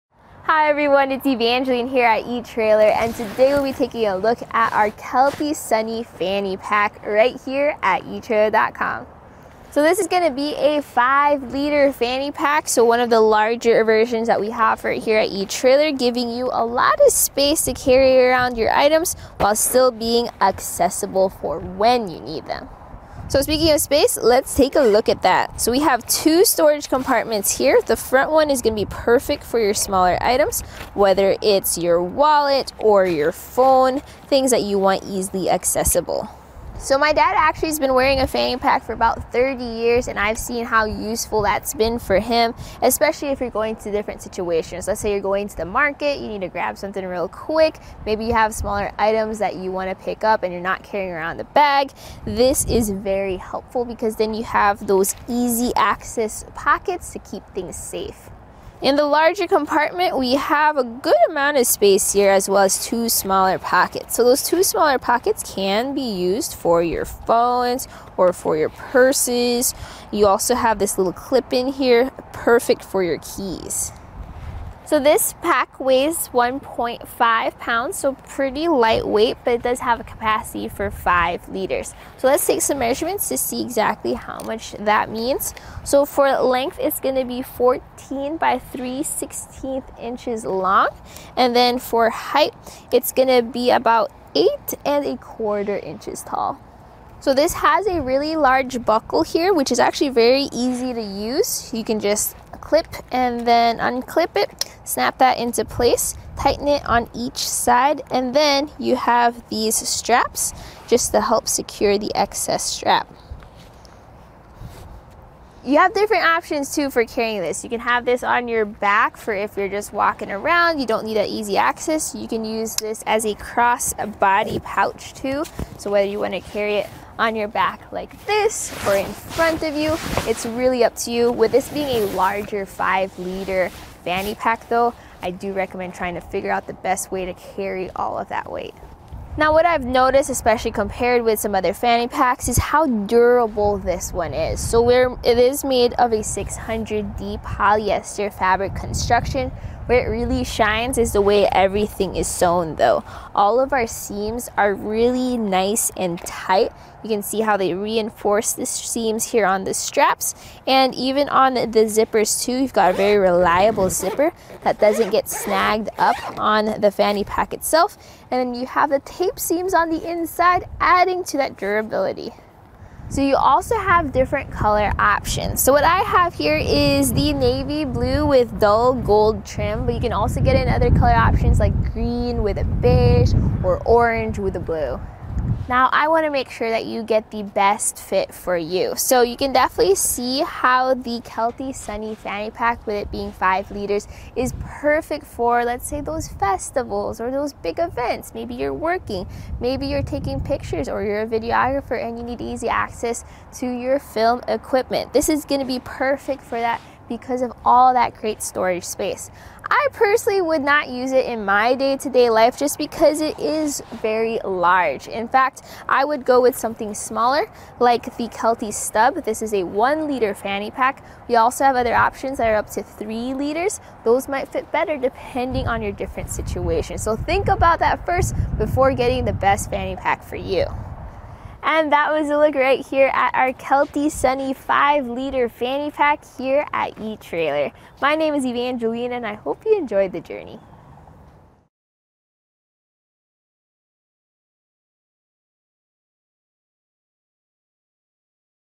etrailer | Feature Review Kelty Sunny Fanny Pack
Hi everyone, it's Evangeline here at etrailer, and today, we'll be taking a look at our Kelty Sunny Fanny Pack right here at etrailer.com. So this is gonna be a five liter fanny pack, so one of the larger versions that we have here at etrailer, giving you a lot of space to carry around your items while still being accessible for when you need them. So speaking of space, let's take a look at that. So we have two storage compartments here, the front one is gonna be perfect for your smaller items, whether it's your wallet or your phone, things that you want easily accessible. So my dad actually has been wearing a fan pack for about 30 years, and I've seen how useful that's been for him, especially if you're going to different situations, let's say you're going to the market, you need to grab something real quick, maybe you have smaller items that you wanna pick up and you're not carrying around a bag, this is very helpful, because then you have those easy access pockets to keep things safe. In the larger compartment, we have a good amount of a space here as well as two smaller pockets.

So those two smaller pockets can be used for your phones or for your purses, you also have this little clip in here, perfect for your keys. So this pack weighs 1.5 pounds, so pretty lightweight, but it does have a capacity for five liters. So let's take some measurements to see exactly how much that means. So for length, it's gonna be 14 3/16 inches long, and then for height, it's gonna be about 8 1/4 inches tall. So this has a really large buckle here, which is actually very easy to use, you can just clip and then unclip it, snap that into place, tighten it on each side, and then you have these straps just to help secure the excess strap.

You have different options too for carrying this, you can have this on your back for if you're just walking around, you don't need the easy access, you can use this as a crossbody pouch too, so whether you wanna carry it on your back like this or in front of you, it's really up to you. With this being a larger five liter fanny pack though, I do recommend trying to figure out the best way to carry all of that weight. Now, what I've noticed, especially compared with some other fanny packs, is how durable this one is, it is made of a 600D polyester fabric construction. Where it really shines is the way everything is sewn though, all of our seams are really nice and tight, you can see how they reinforce the seams here on the straps, and even on the zippers too, you've got very reliable zipper that doesn't get snagged up on the fanny pack itself, and then you have the taped seams on the inside adding to that durability. So you also have different color options, so what I have here is the navy blue with dull gold trim, but you can also get in other color options like green with a beige or orange with a blue.

Now, I wanna make sure that you get the best fit for you, so you can definitely see how the Kelty Sunny Fanny Pack with it being five liters is perfect for, let's say, those festivals or those big events, maybe you're working, maybe you're taking pictures or you're a videographer and you need the easy access to your film equipment, this is gonna be perfect for that, because of all that great storage space. I personally would not use it in my day-to-day life just because it is very large. In fact, I would go with something smaller like the Kelty Stub, this is a one liter fanny pack, we also have other options that are up to three liters, those might fit better depending on your different situation, so think about that first before getting the best fanny pack for you. And that was look right here at our Kelty Sunny five liter fanny pack here at etrailer. My name is Evangeline, and I hope you enjoyed the journey..